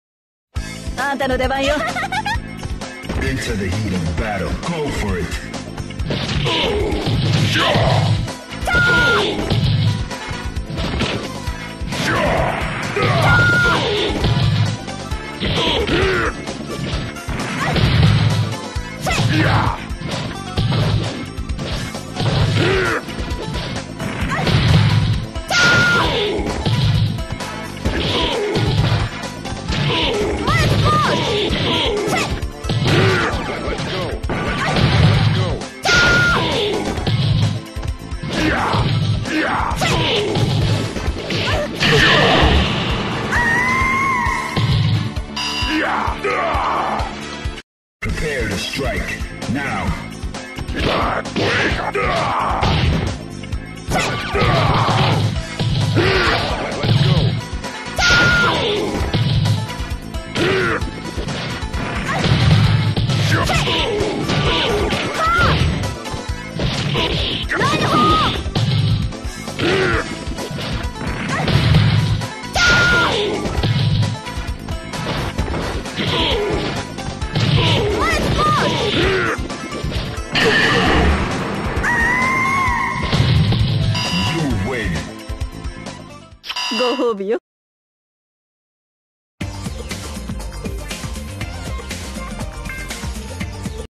I love whooping Elana's beautiful ass......again!
Pervert moment:
If you notice, everytime Elena is caught in Hugo's Shootdown Backbreaker, Her beautiful booty is on the back of Hugo's neck. Just watch it and you'll see what I'm talking about.